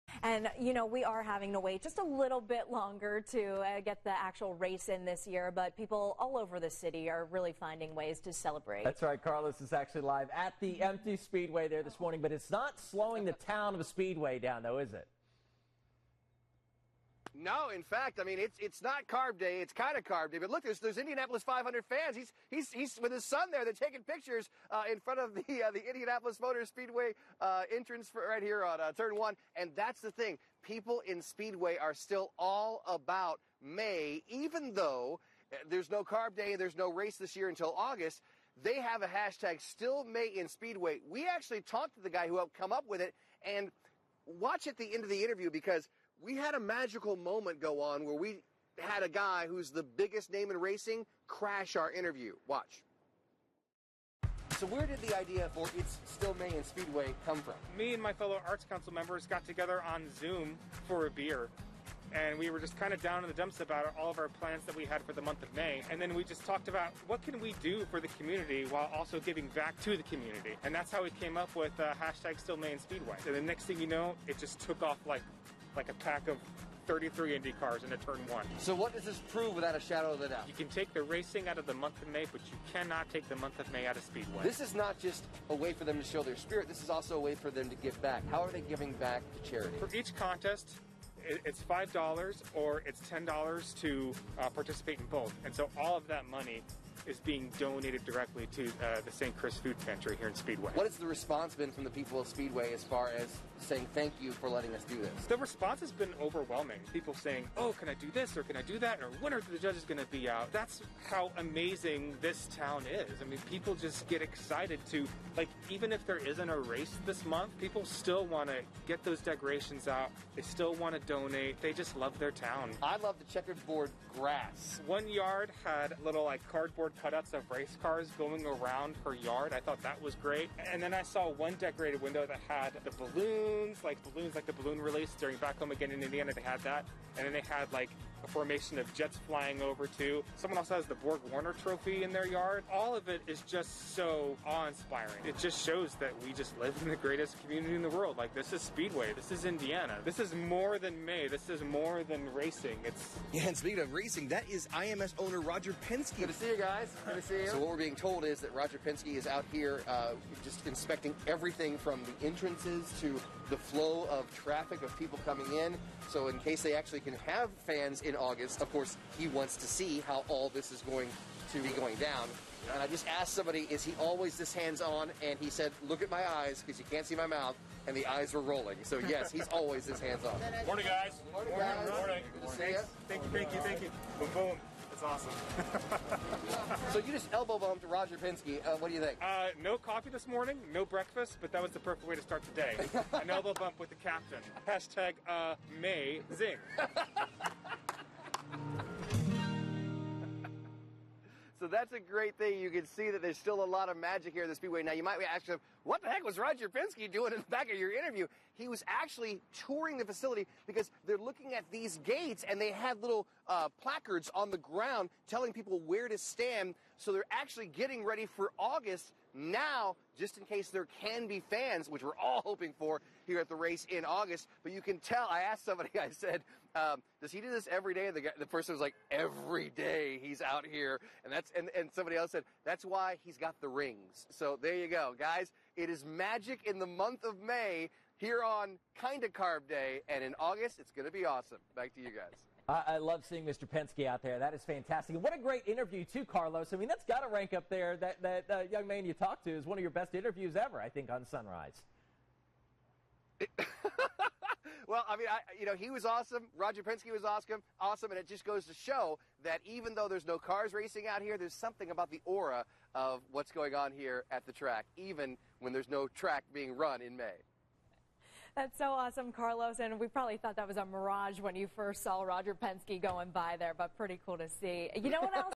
Home decorating contest proves its
The hashtag StillMayInSpeedway is not just about decorating houses — it’s about giving back to those in need in the city of Speedway.